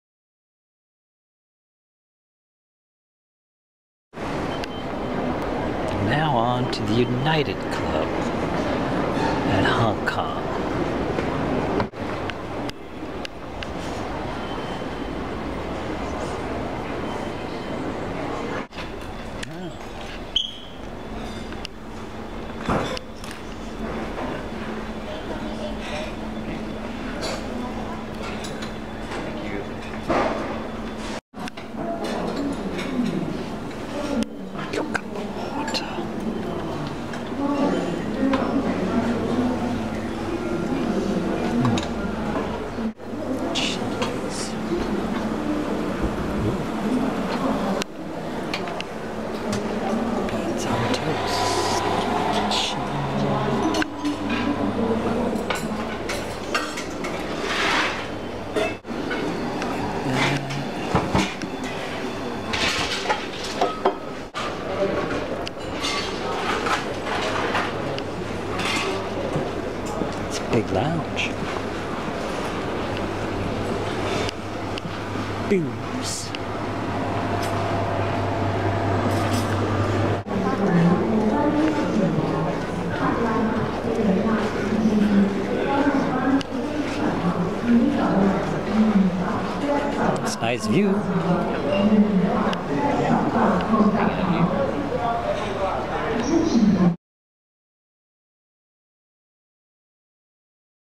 United Club Hong Kong Airport
Spacious and open to the terminal below, the United Club is a surprising good Star Alliance Gold option in Hong Kong Airport. The  f breakfast food selection was sure to please. There's booze to be had (if you are there later in the day). And the views out to the airport to watch the planes or to the terminal to watch flyers are great.